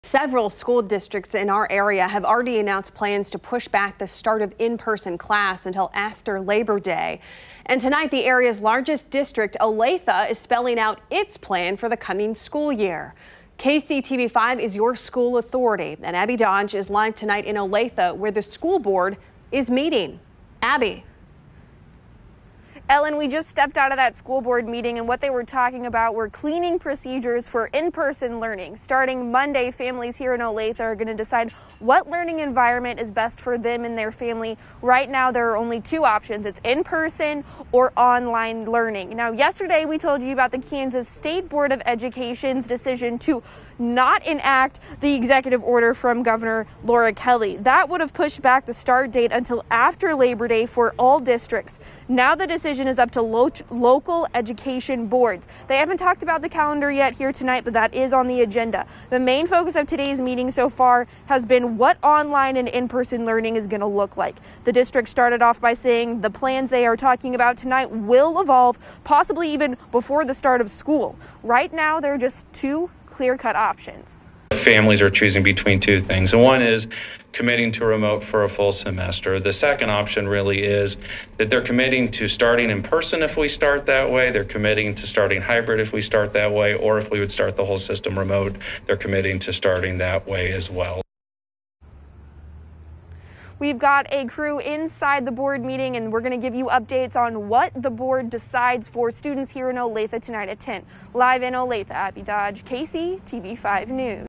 Start date for Olathe students set at Sept. 8
Tonight, the area's largest district -- Olathe -- is spelling out its plan for the coming school year.